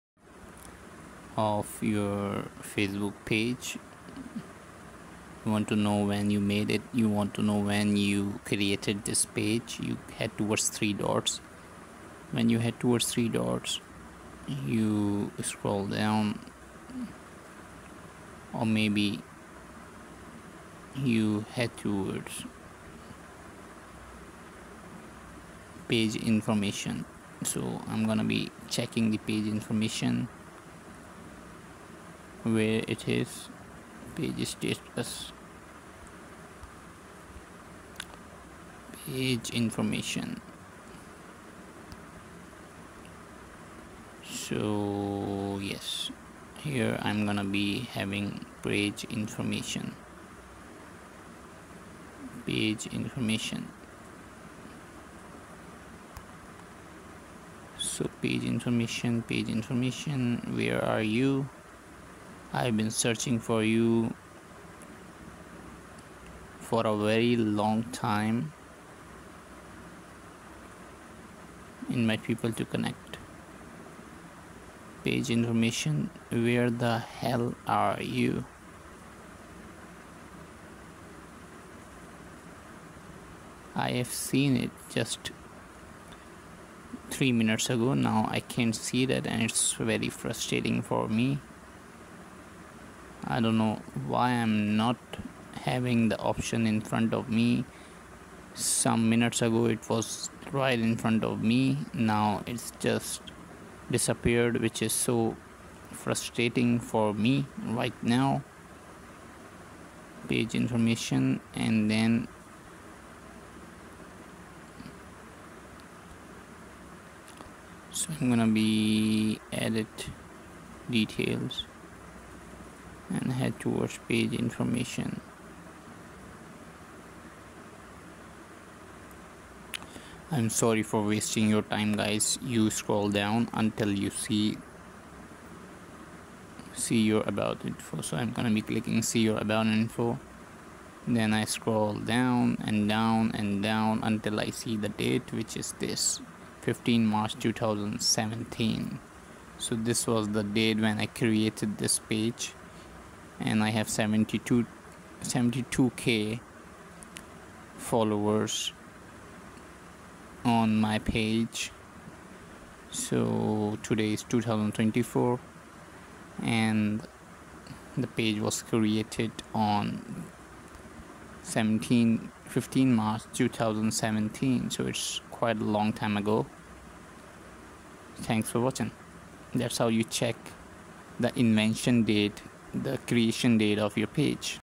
how to check creation date of your FACEBOOK PAGE?
Invention date and which day you made your Facebook page?